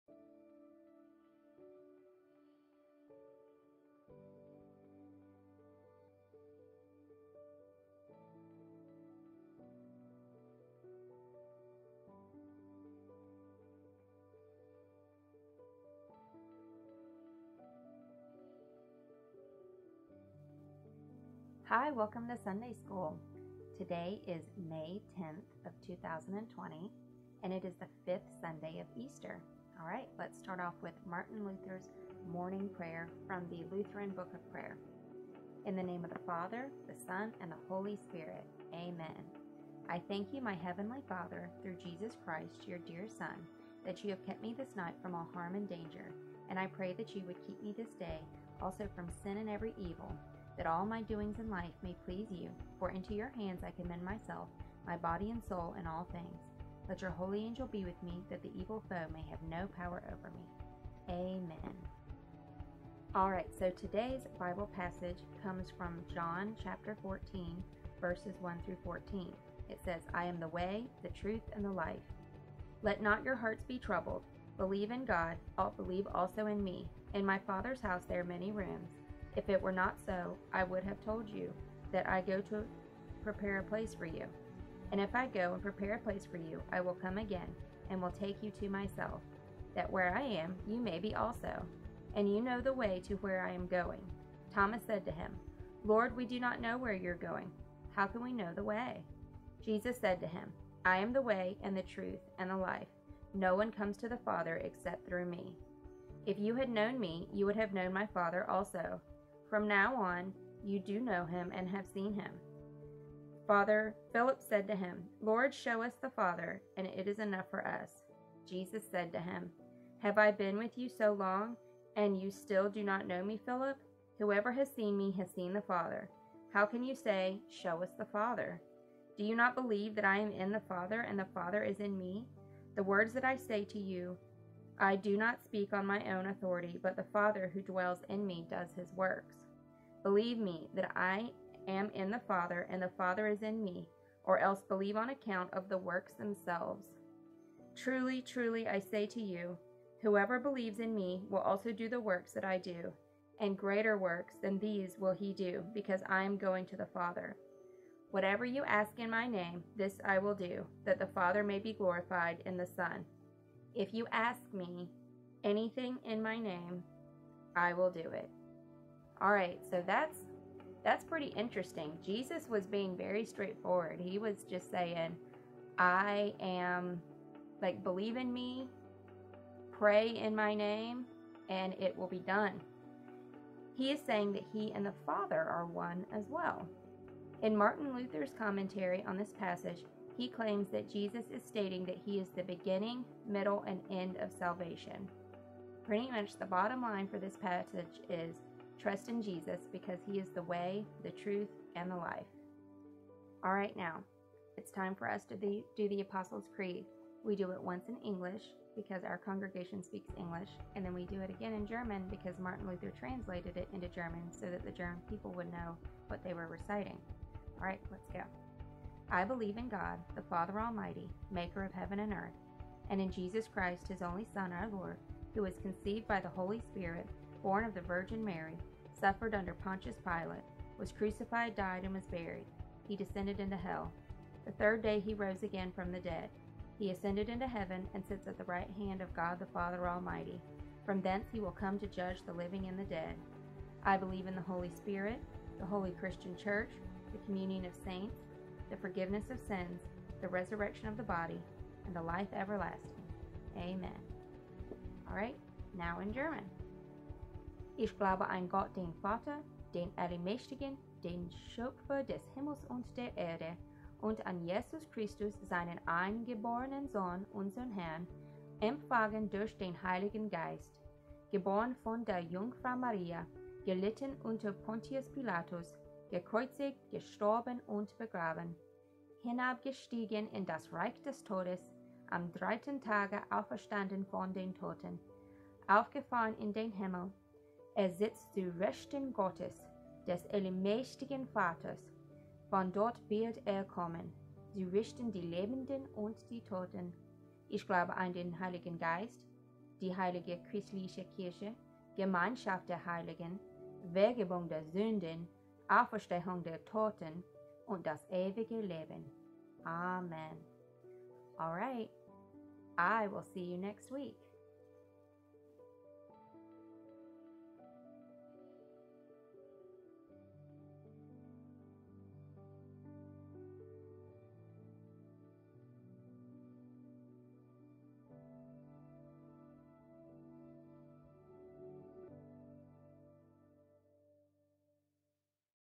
Children's Message 5 10 20
Hey Kids! Mrs. Rebecca has a special message for you today! Enjoy!